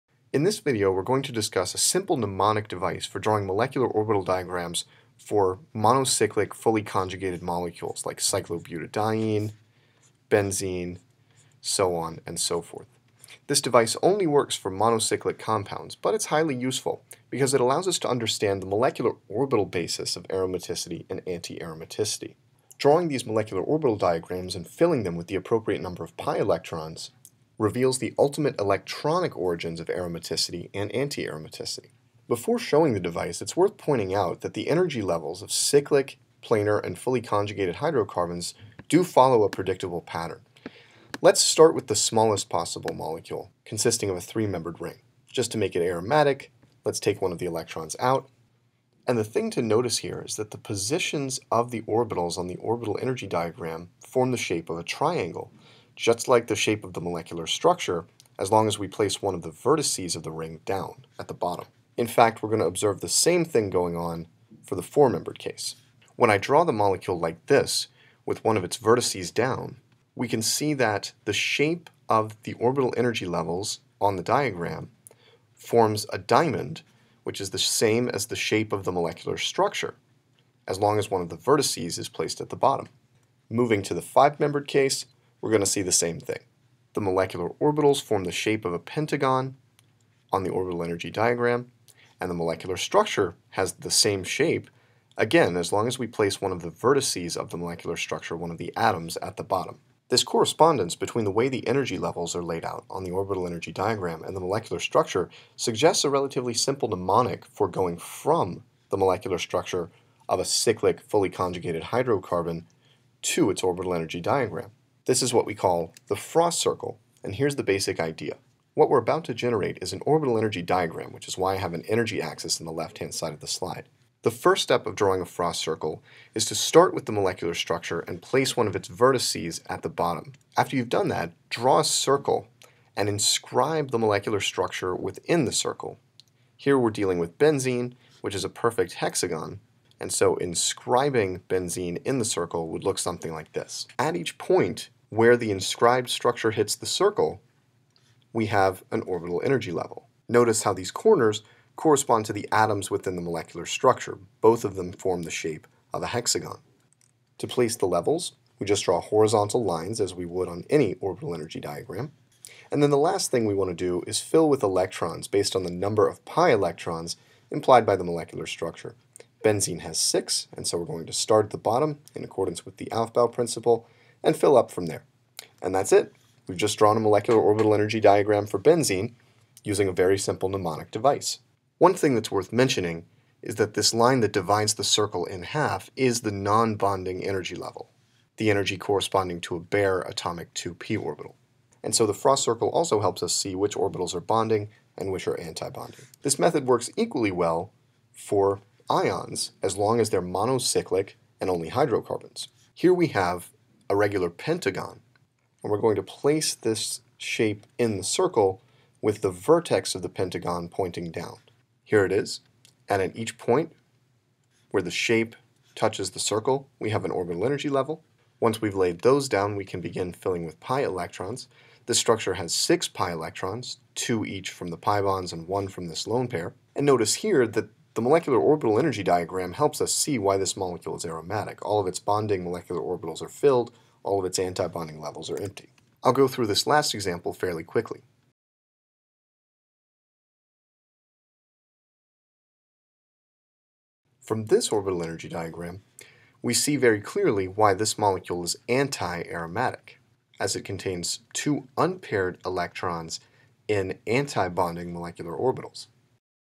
31.06 The Frost Circle
A mnemonic device for drawing orbital energy diagrams for monocyclic fully conjugated hydrocarbons.